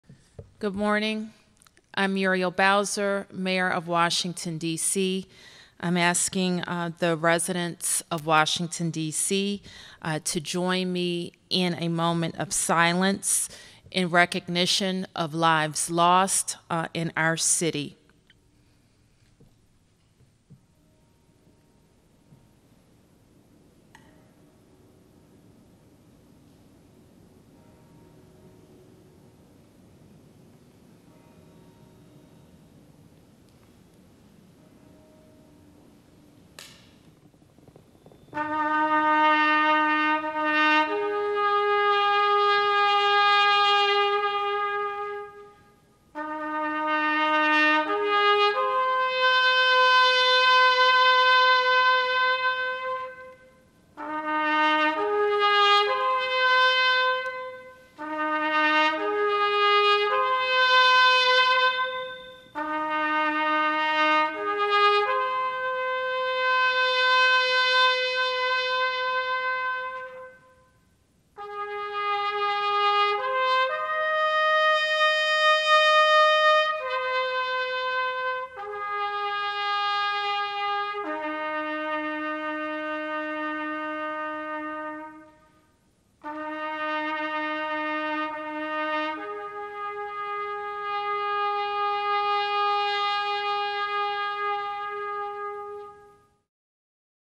DC Mayor Bowser leads city in moment of silence
The Mayor of Washington DC led the District in a moment of silence Friday at Gonzaga College High School to honor those who have died in the coronavirus pandemic. (April 10)